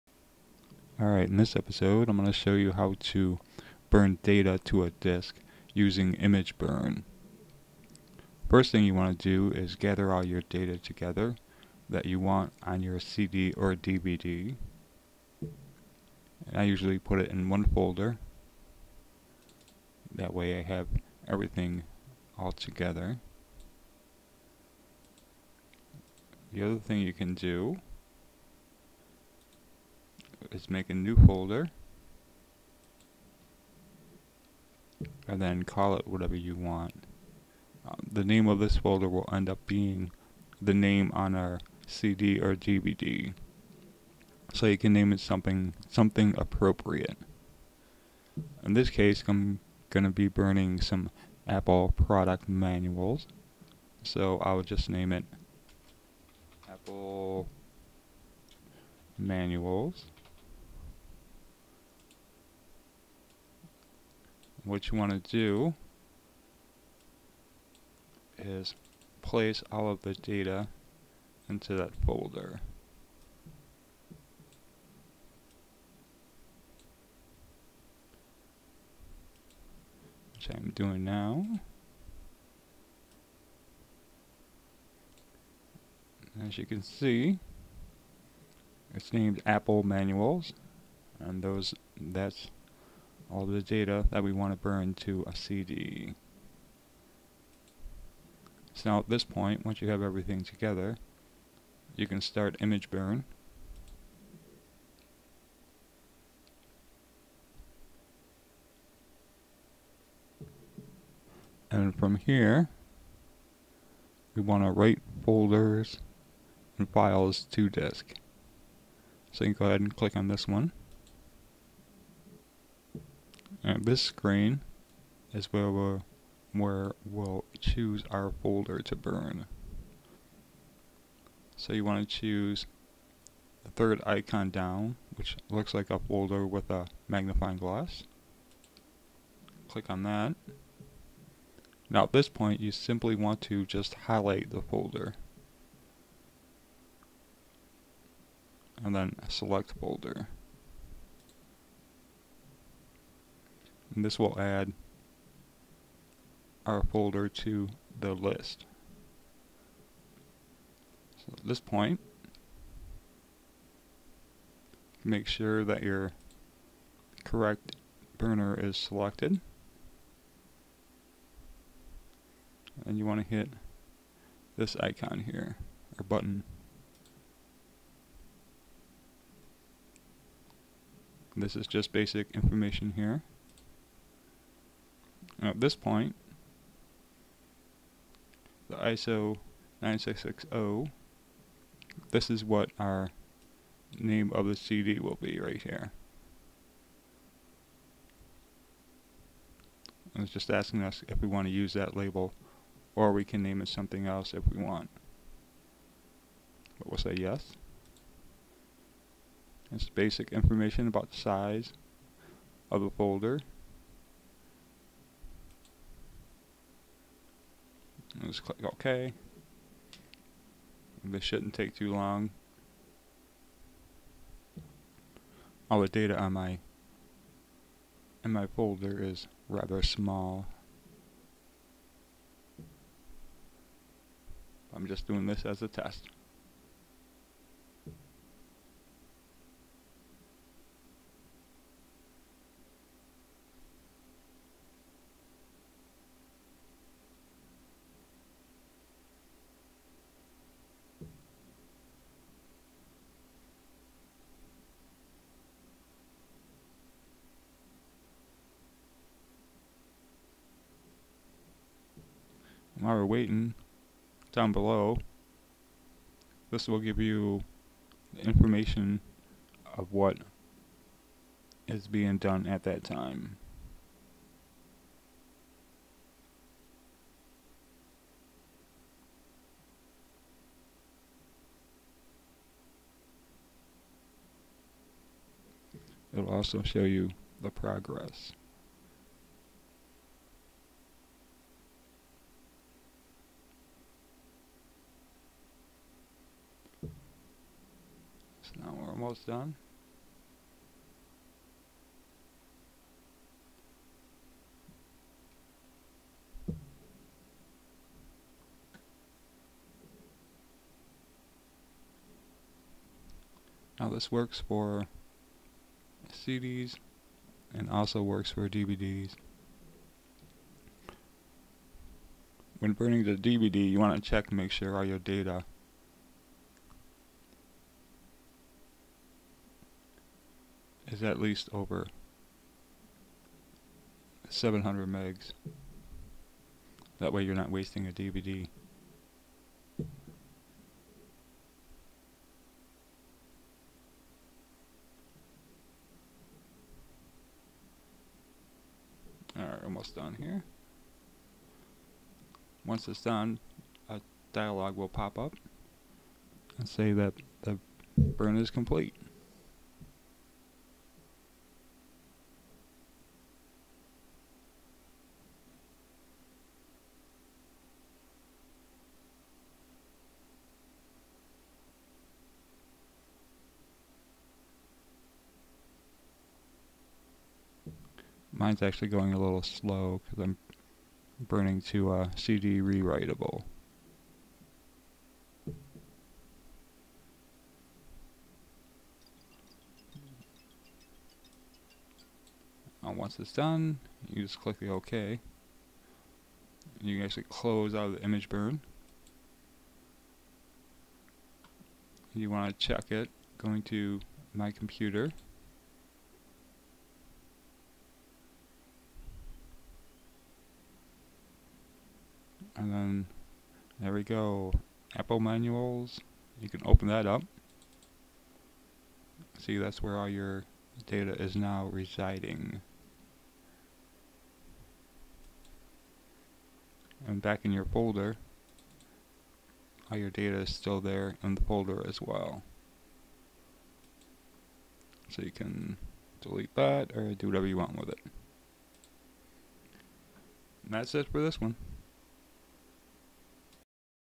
IMGBURN Burning Files to Disc
Just a basic HOW TO of burning data to a disc...